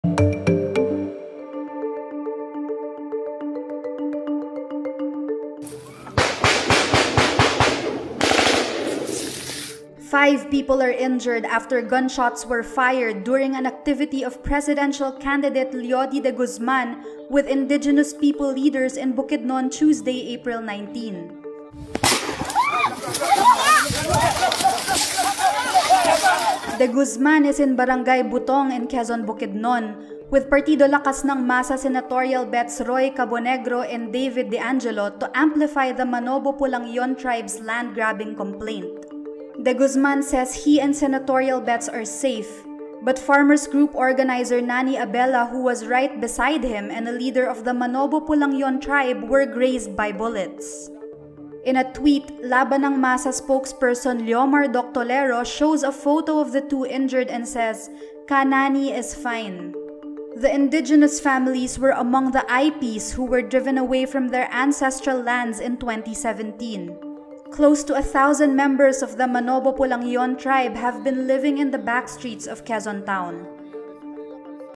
5 hurt as shots fired during Bukidnon activity of De Guzman
More videos on Rappler Five people are injured after gunshots were fired during an activity of presidential candidate Leody de Guzman with indigenous people leaders in Bukidnon Tuesday, April 19.

Full story Follow Rappler for the latest news in the Philippines and around the world.